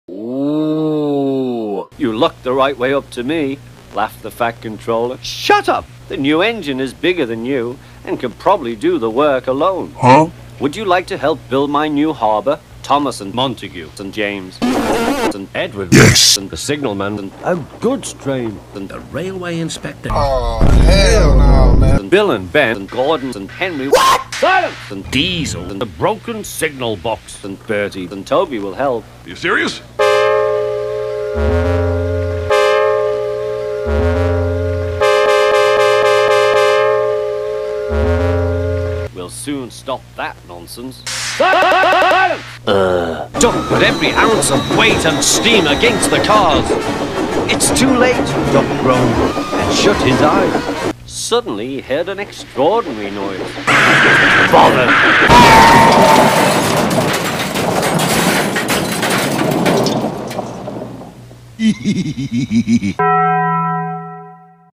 The O Faces: REIMAGINED | Dankelodeon UK
After over a decade (or around 12 years), a YouTube Poop video titled "The O Faces" has been reimagined for the new & modern YouTube Poop generation!

Note: Some parts may not be true to the original ¯⁠\⁠_⁠(⁠ツ⁠)⁠_⁠/⁠¯

WARNING: This video contains loud audio and flashing lights.